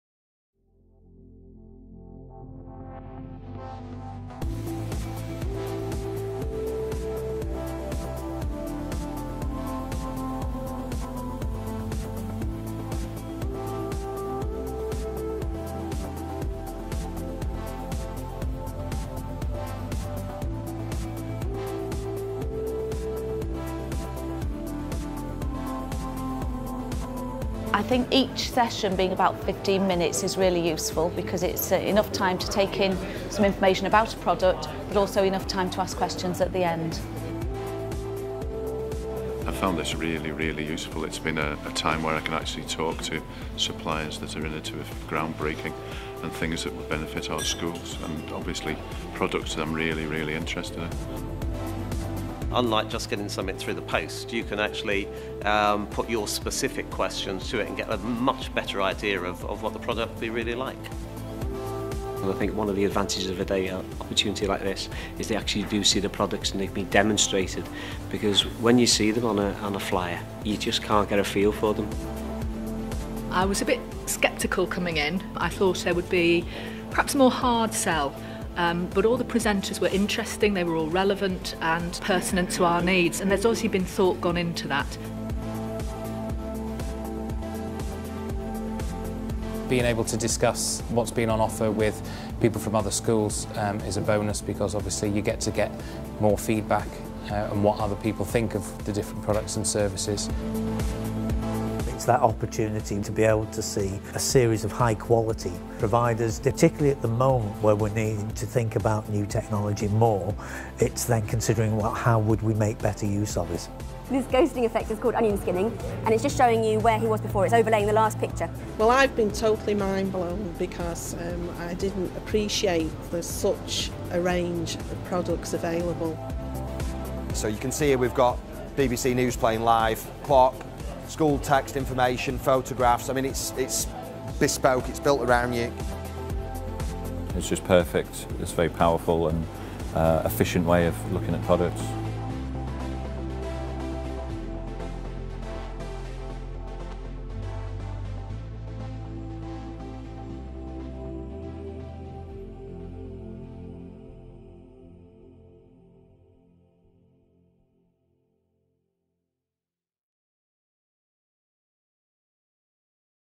Ideas and Resources for Creative Computing
An after-school event organised by Innovate My School, hosted at Ashton on Mersey Teaching School.